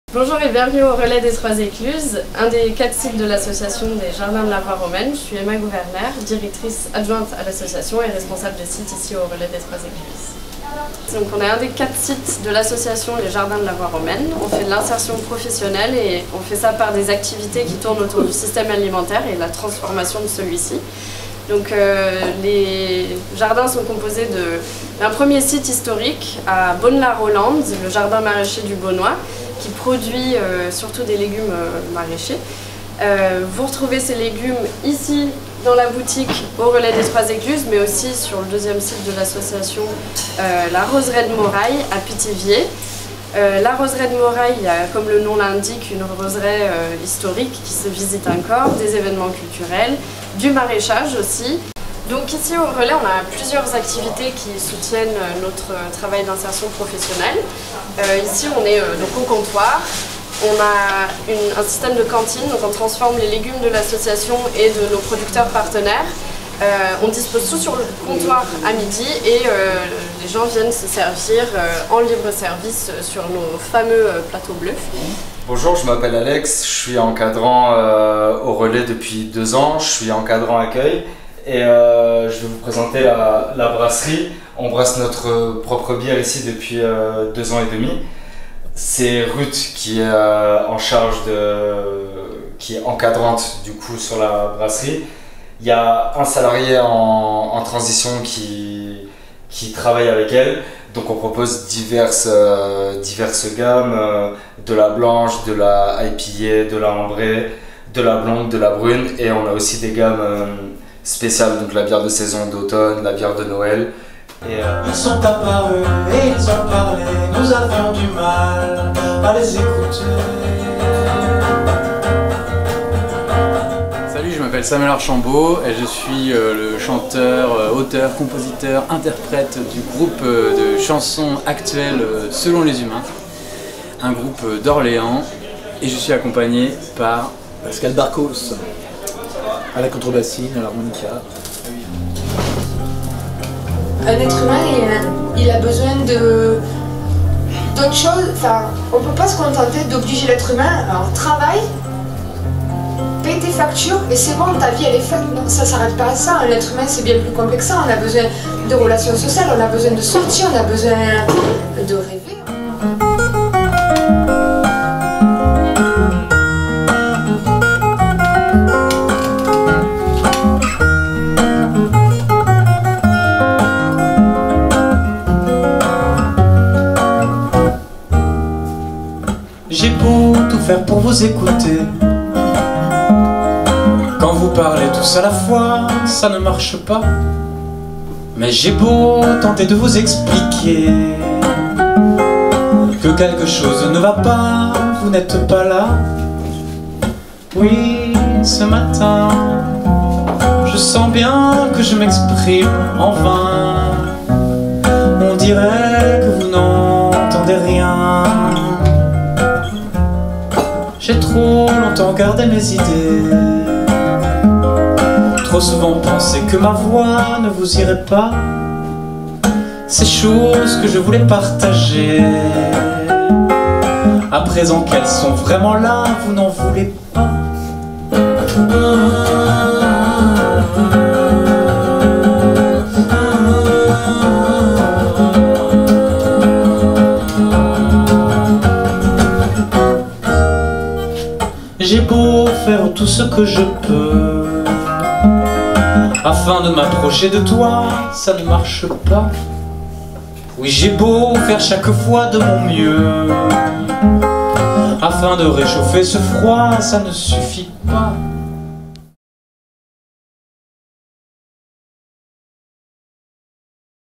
Relais des trois écluses à Grignon
Tiers lieu rural, solidaire et nourricier

Bienvenue au Relais des Trois écluses, à Grignon, Vieilles-maisons-sur-joudry.

Ouvert du mercredi au dimanche, venez vous restaurer, prendre un brunch, un thé ou des gâteaux. Ici la cuisine est faite maison, à partir de produits locaux (moins de 100km) et issus d’une agriculture biologique ou durable.

Le Relais est un lieu d’insertion professionnelle, il allie une action sociale à une offre de services diversifiée, on s’y ressource, on s’y reconstruit…